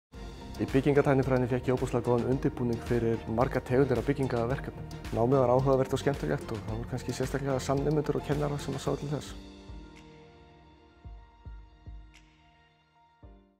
Byggingartæknifræði við HR
Byggingartæknifræðingar taka þátt í mótun umhverfis okkar með hönnun, framkvæmdum og rekstri mannvirkja. Þar að auki fást þeir við verkefni tengd umhverfis- og skipulagsmálum: umhverfismati framkvæmda og þróun á umhverfisvænum lausnum fyrir byggingariðnaðinn.